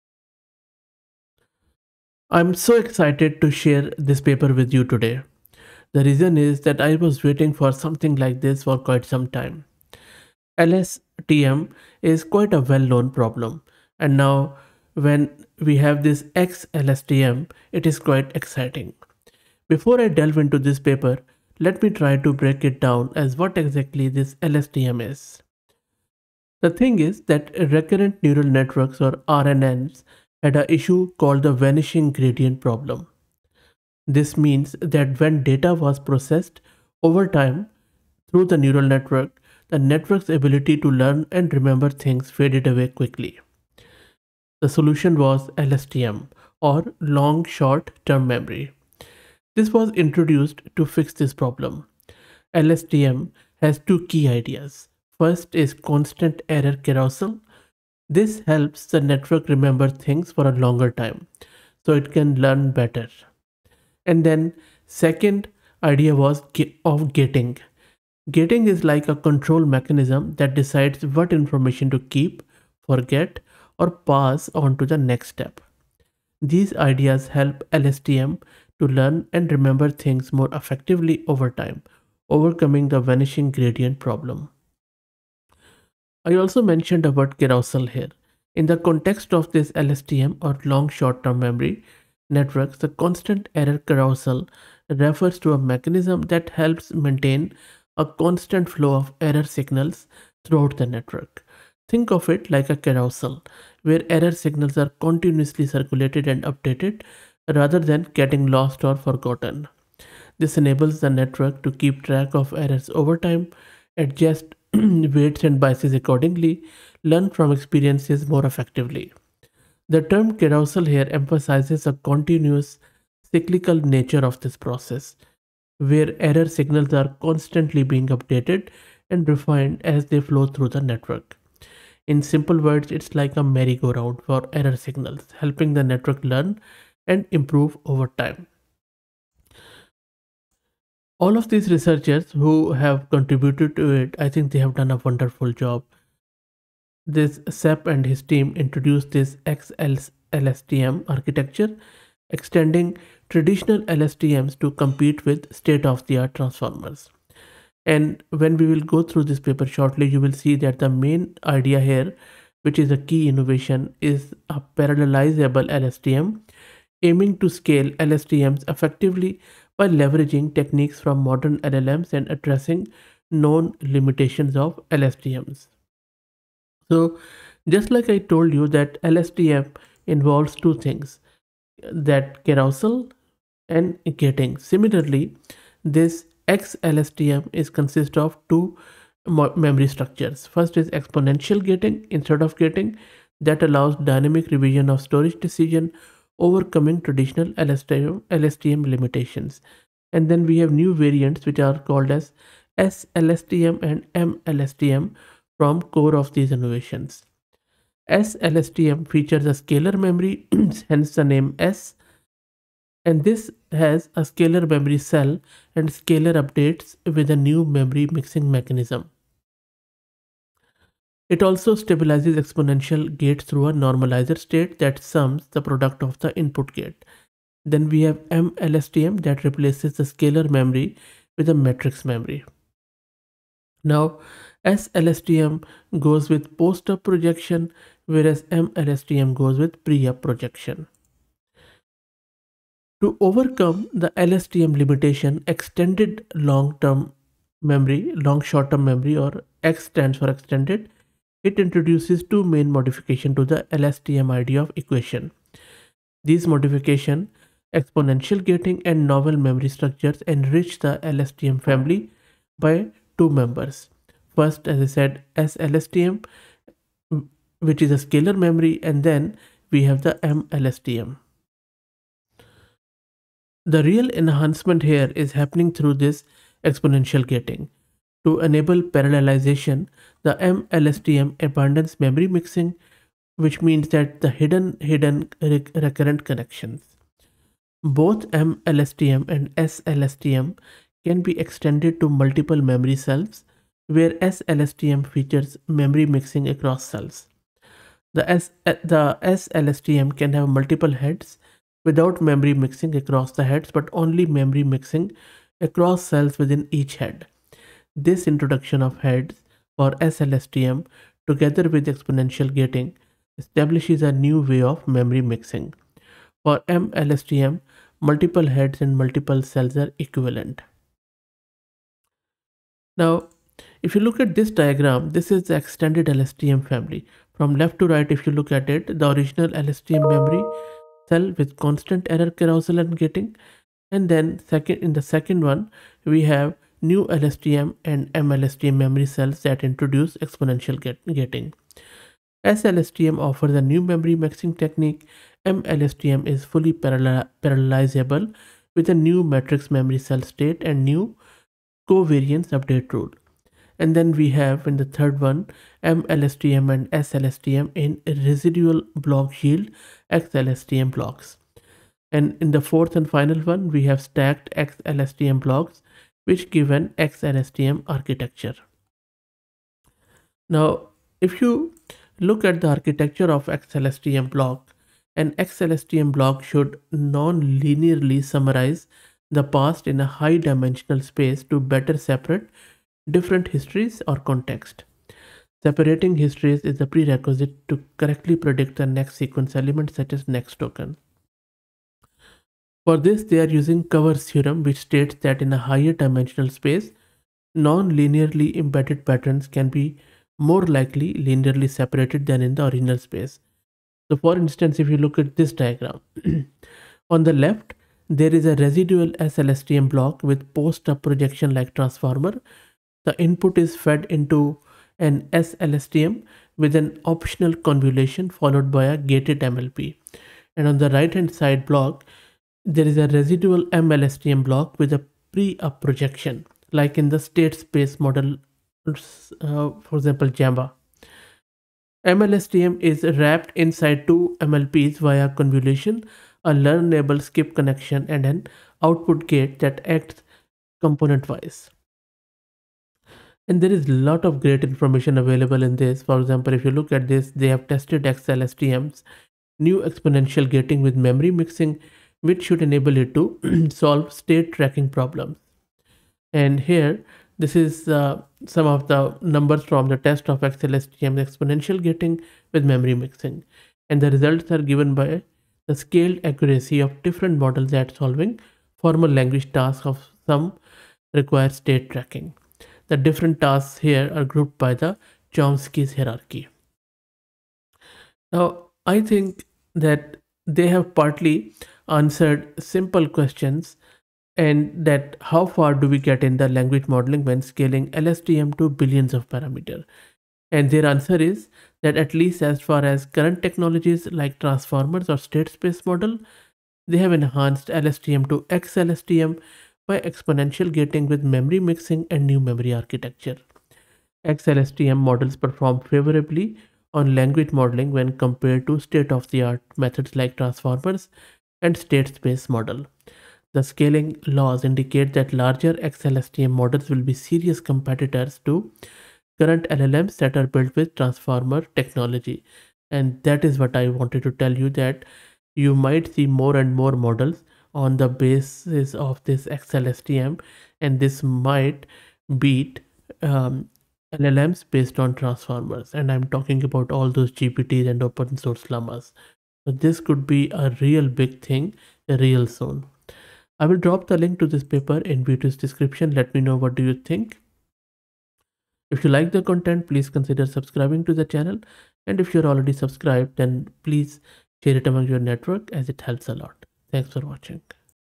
xLSTM Models Might Beat Transformer LLMs
This video demystifies xLSTM which is Extended Long Short-Term Memory and introduces key ideas. The scaling laws indicate that larger xLSTM models will be serious competitors to current LLMs that are built with the Transformer technology.